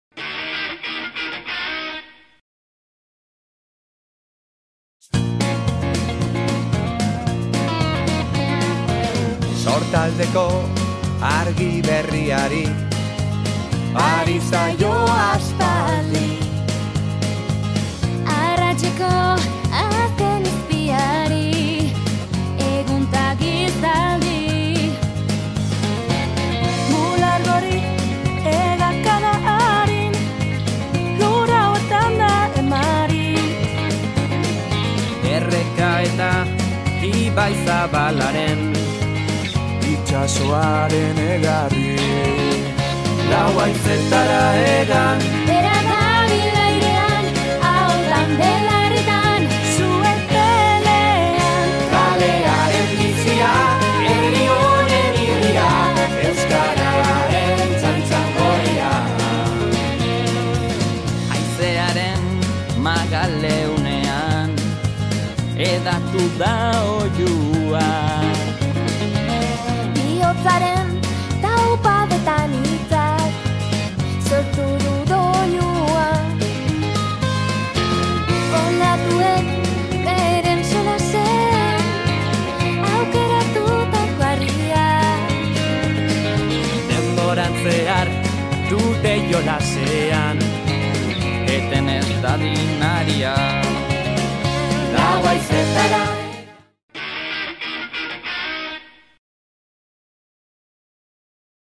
Euskararen eguna 2018 Arteagabeitia Zuazon
Hona hemen, Arteagabeitia Zuazon ospatu den Euskararen eguneko irudien laburpena